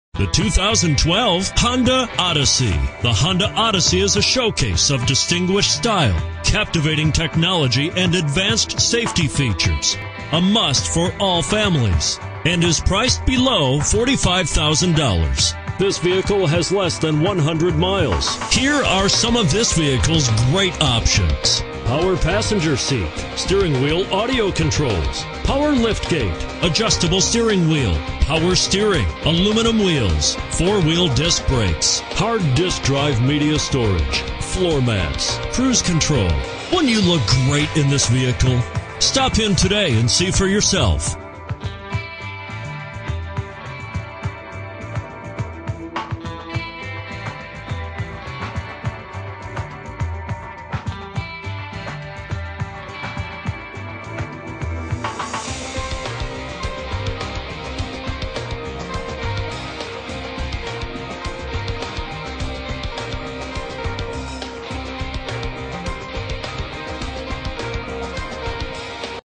2012 HONDA ODYSSEY Boulder, CO 124054
2012 HONDA ODYSSEY Boulder, CO
Stock #124054

Fisher Honda
6025 Arapahoe Road

Don't miss this 2012 Honda Odyssey. It's equipped with automatic transmission and features a White Diamond Pearl exterior. With 12 miles, you'll want to take this car home. Make a great choice today.